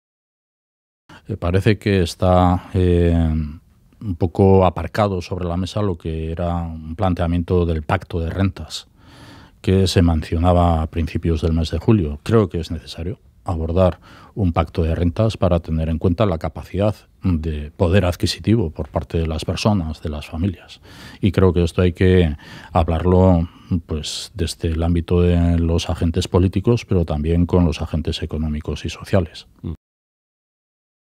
Urkullu apuesta por retomar el debate "aparcado" del pacto de rentas
El lehendakari, Iñigo Urkullu, ha apostado por retomar el debate "aparcado" sobre el pacto de rentas, que ve "necesario", y, en este sentido, ha apelado a "hablarlo desde el ámbito de los agentes políticos pero también con los agentes económicos y sociales".

(Fuente: Irekia)